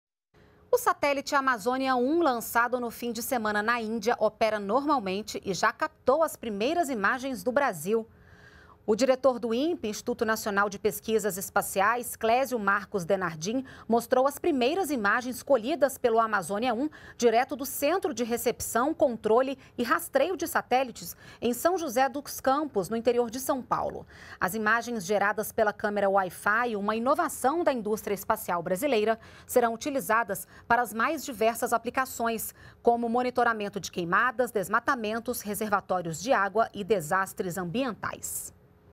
Amazonia-1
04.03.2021 - O satélite Amazonia-1, lançado no fim de semana, na Índia, opera normalmente e já captou as primeiras imagens do Brasil.